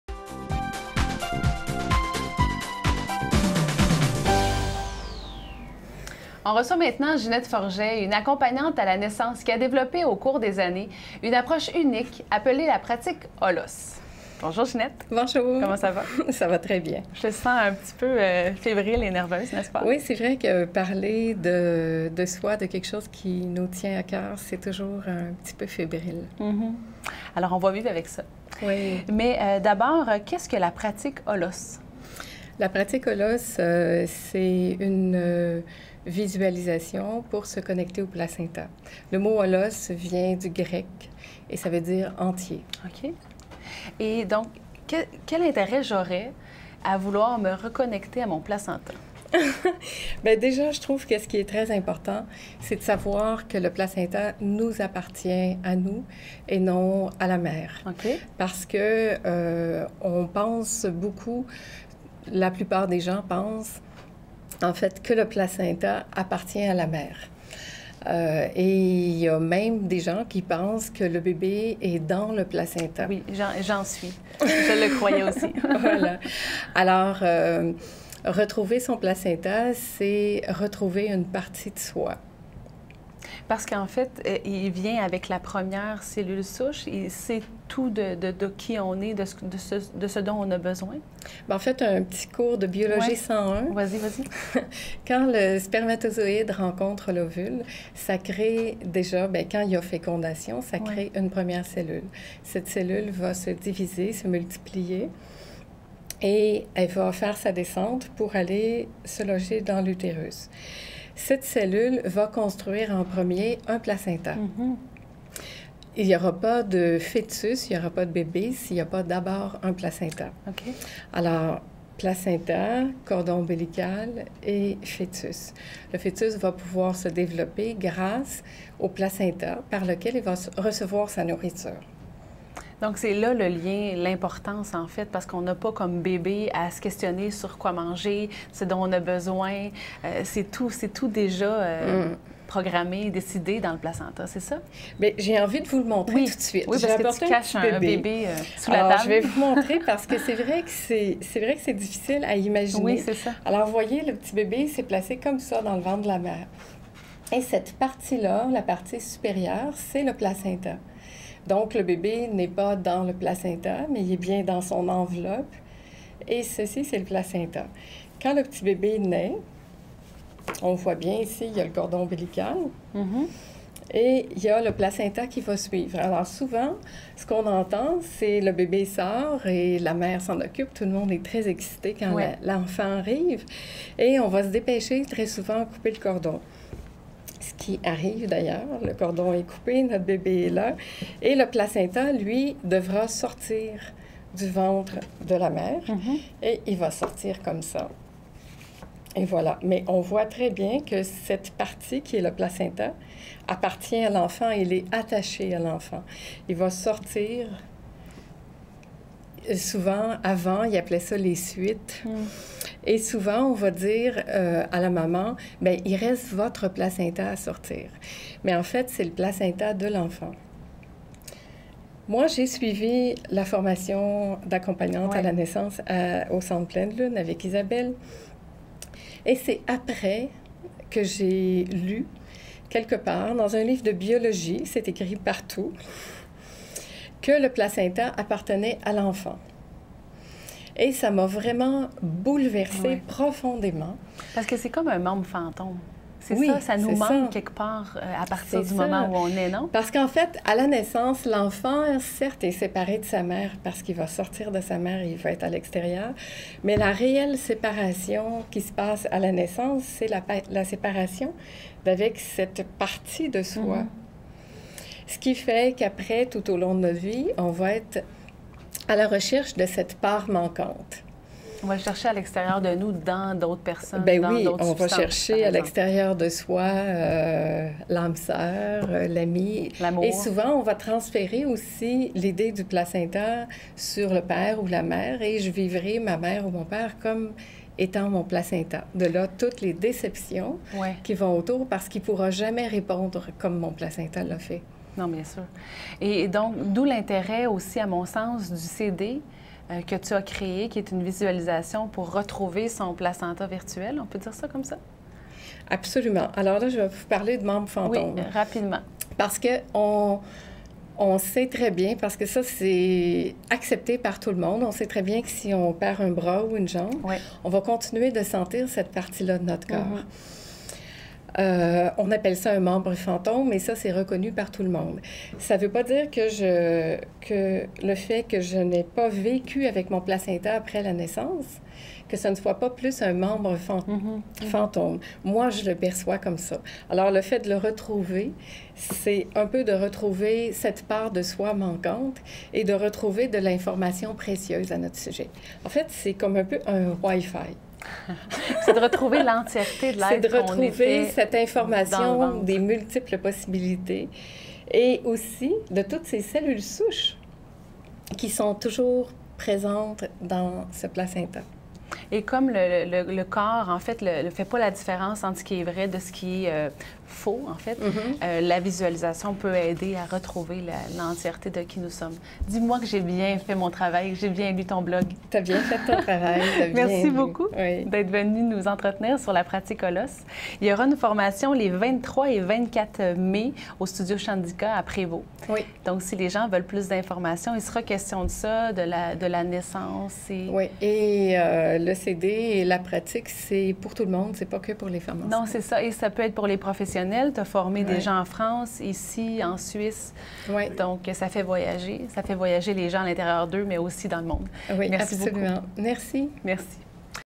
La Pratique HOLOS
Julie reçoit : Ginette Forget, fondatrice et formatrice "La Pratique HOLOS" (placenta)